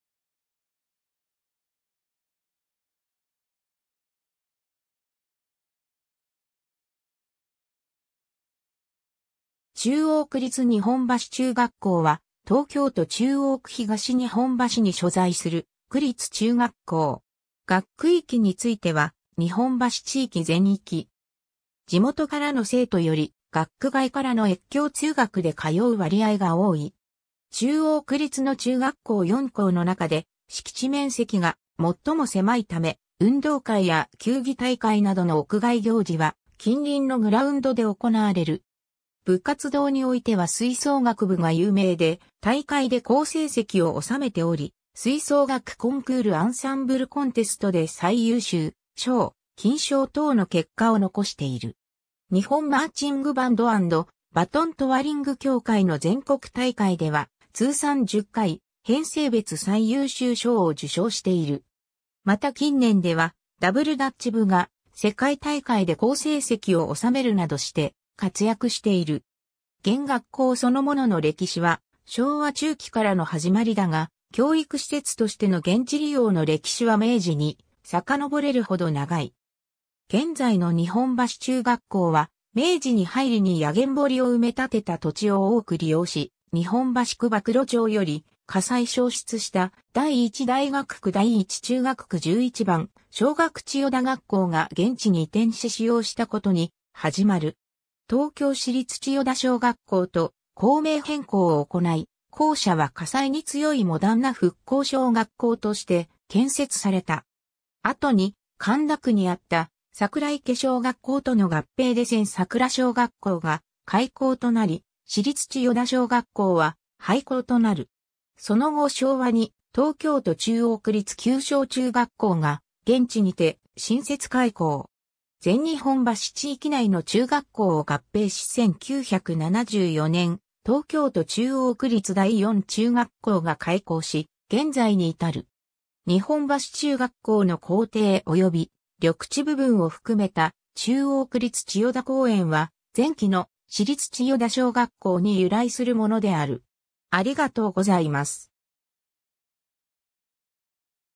中央区立日本橋中学校
中央区立日本橋中学校（ちゅうおうくりつ にほんばしちゅうがっこう）は、東京都中央区東日本橋に所在する区立中学校。
 学区域については日本橋地域全域。
地元からの生徒より学区外からの越境通学で通う割合が多い。
中央区立の中学校4校の中で敷地面積が最も狭いため、運動会や球技大会などの屋外行事は近隣のグラウンド（浜町公園等）で行われる。
 部活動においては吹奏楽部が有名で、大会で高成績を収めており、吹奏楽コンクール・アンサンブルコンテストで最優秀賞・金賞等の結果を残している。
日本マーチングバンド＆バトントワリング協会の全国大会では通算10回、編成別最優秀賞を受賞している。
また近年ではダブルダッチ部が世界大会で高成績を収めるなどして活躍している。
 現学校そのものの歴史は昭和中期からの始まりだが、教育施設としての現地利用の歴史は明治に遡れる程長い。
 現在の日本橋中学校は、明治に入りに薬研堀を埋め立てた土地を多く利用し、日本橋区馬喰町より火災焼失した『第一大学区第一中学区十一番小学千代田学校』が現地に移転し使用したことに始まる。
『東京市立千代田小学校』と校名変更を行い、校舎は火災に強いモダンな復興小学校として建設された。
 後に神田区にあった桜池小学校との合併で千桜小学校が開校となり、市立千代田小学校は廃校となる。
 その後昭和に『東京都中央区立久松中学校』が現地にて新設開校。
全日本橋地域内の中学校を合併し1974年『東京都中央区立第四中学校』が開校し現在に至る。
 日本橋中学校の校庭および緑地部分を含めた『中央区立千代田公園』は、前記の『市立千代田小学校』に由来するものである。